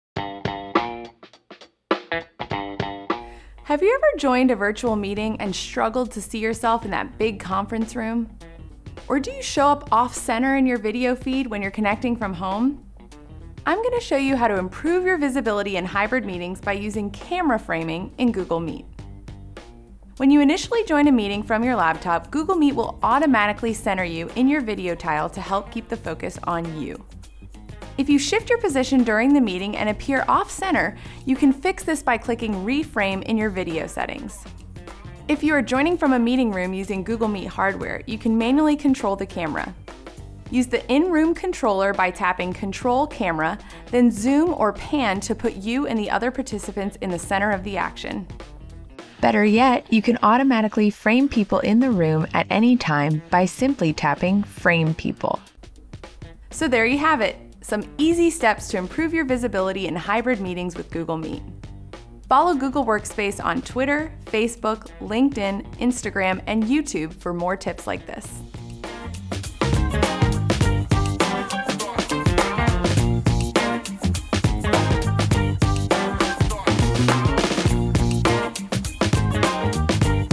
How to use camera framing in Google Meet
Want to center yourself in Google Meet without adjusting your camera? Want to make yourself easier to see in video? In this episode of Hybrid Meeting Tips using Google Workspace, we show how you can adjust the camera framing in Google Meet in one easy step!